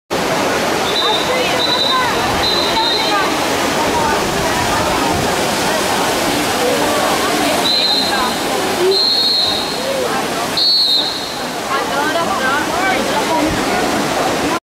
Spectacular Rome, Italy - Fontana di Trevi
Located in the Trevi district of Rome, the Trevi Fountain makes the list for world famous historic sites. As you can see here, it is quite the attraction site. When in Rome, you may like to visit this incredible landmark, which has been standing since the mid 1700's!